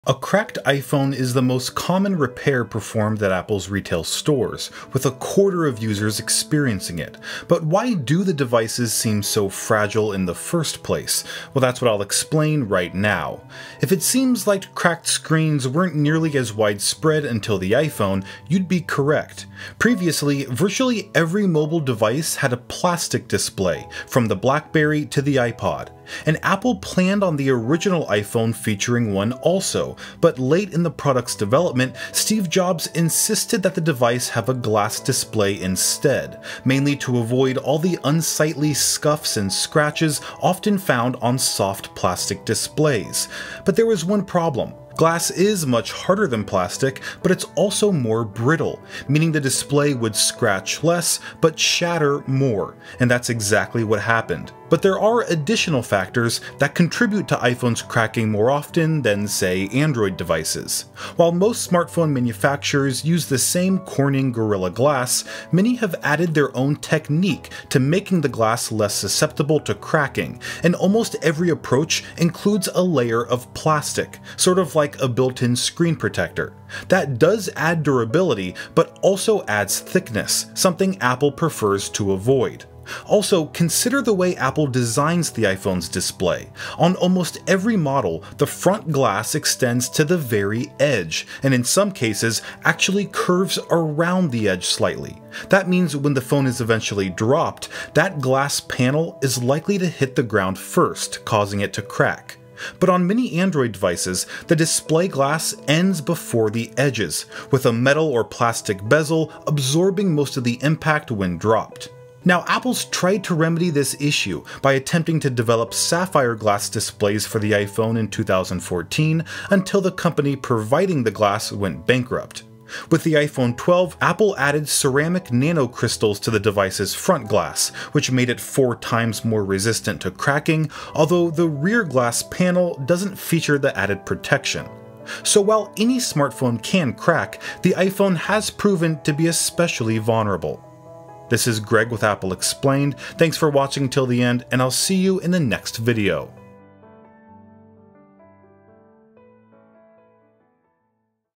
Why iPhones Crack So Easily
A cracked iPhone is the most common repair performed at Apple’s retail stores. With a quarter of users experiencing it. But why do iPhones seem so fragile in the first place? Well that’s what I’ll explain right now.